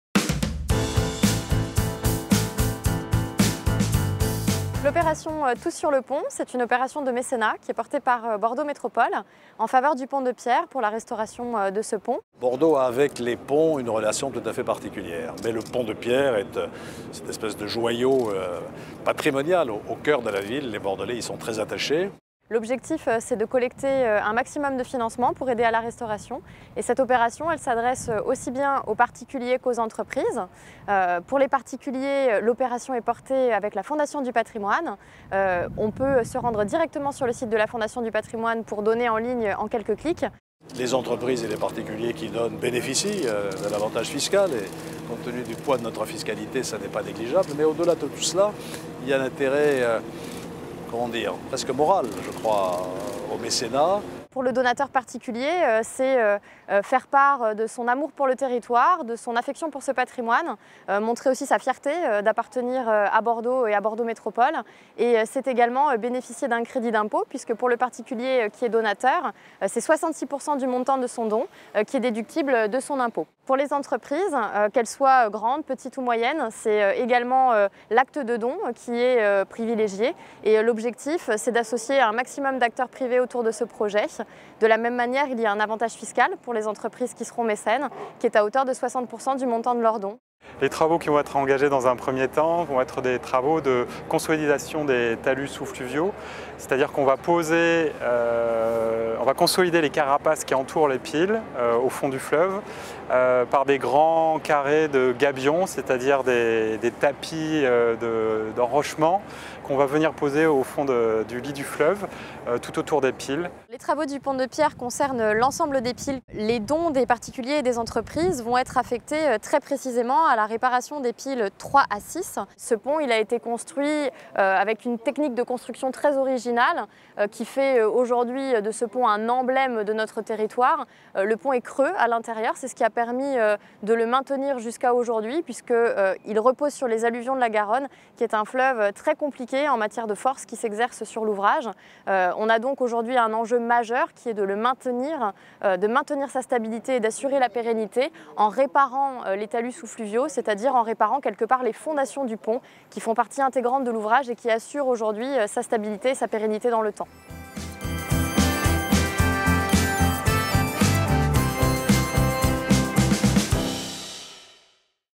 Tous sur le pont ! Le mécénat.
Présentation du mécénat de Bordeaux Métropole pour la restauration du pont de pierre, avec :
Alain Juppé, Président de Bordeaux Métropole, Maire de Bordeaux, ancien Premier ministre,
François Durquety, Adjoint général mobilités, Bordeaux Métropole,
Laura Exposito Del Rio, Responsable mécénat, Bordeaux Métropole.